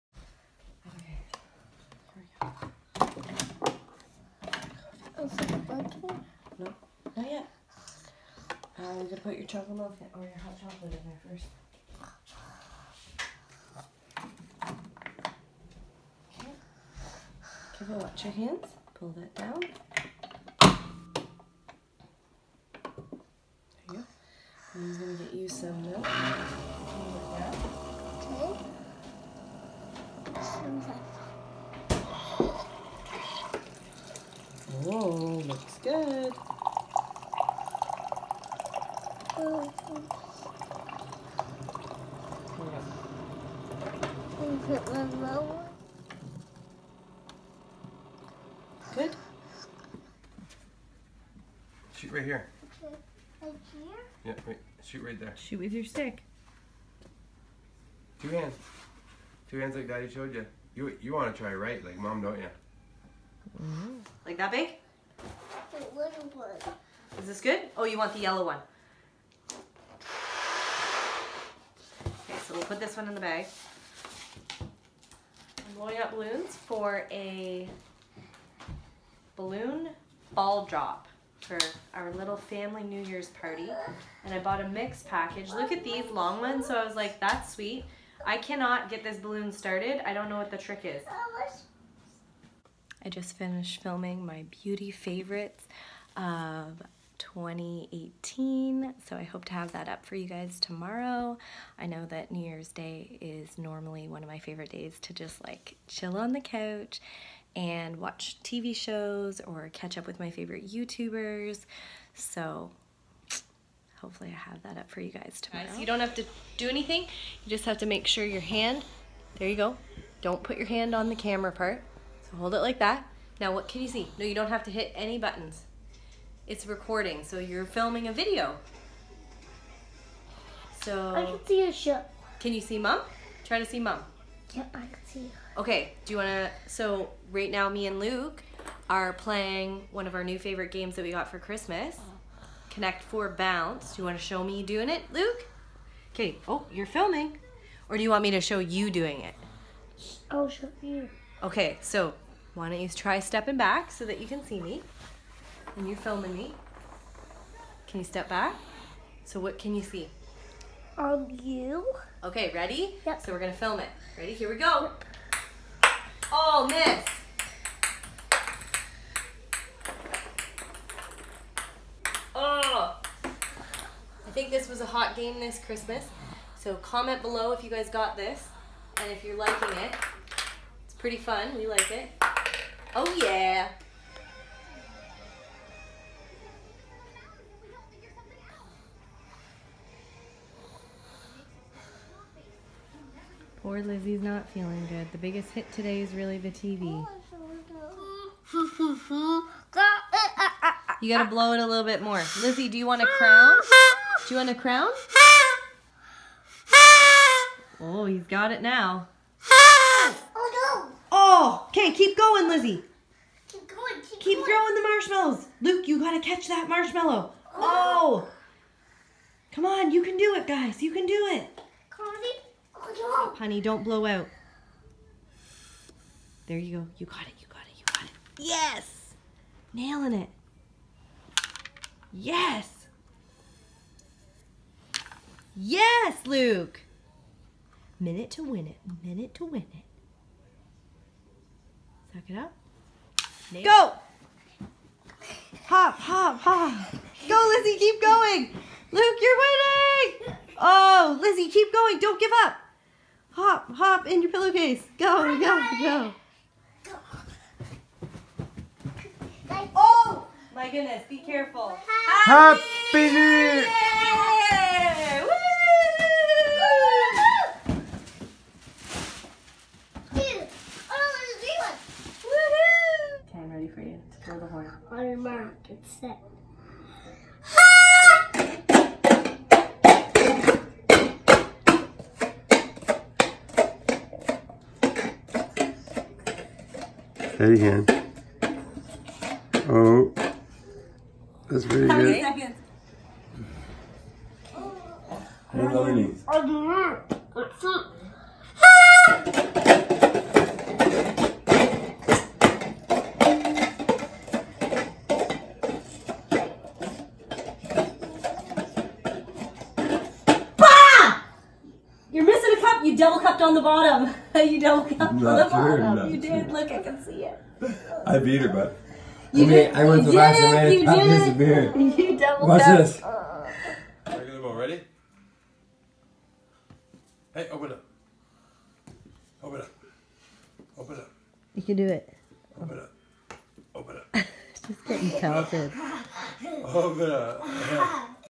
Family New Years Eve Party | Daily Vlog | HAL
We hope you had a wonderful New Year’s Eve! We had a little family party, check out what we did to celebrate 2019.